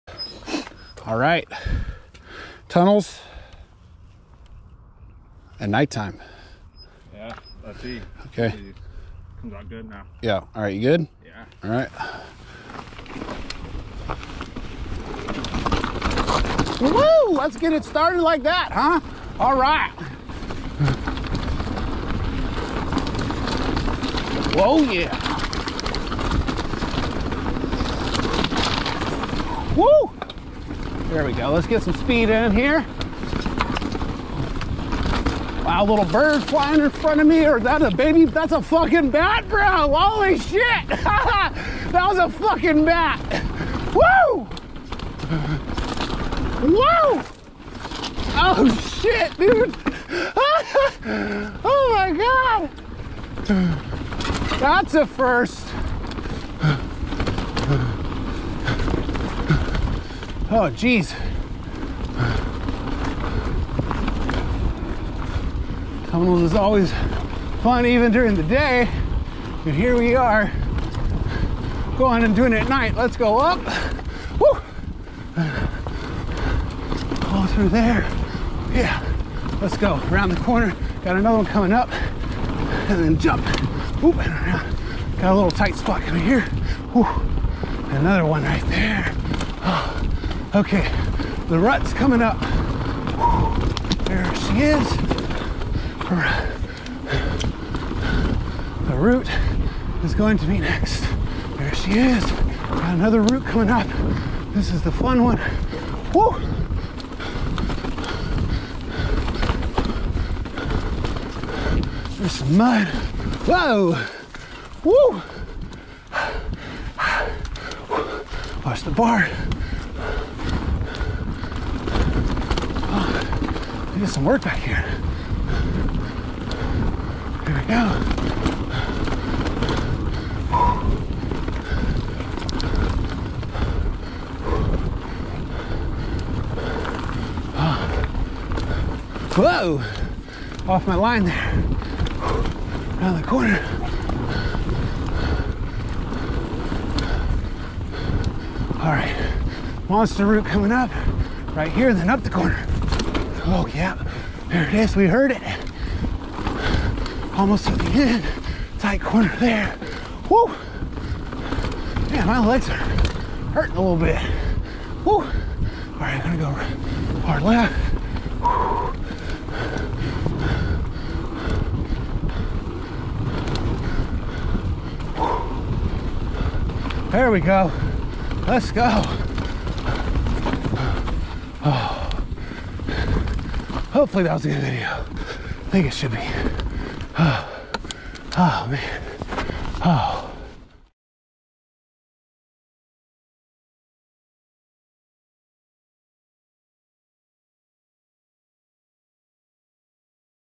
PQ Tunnels at Night
Ian races a bat down Tunnels. Who will win?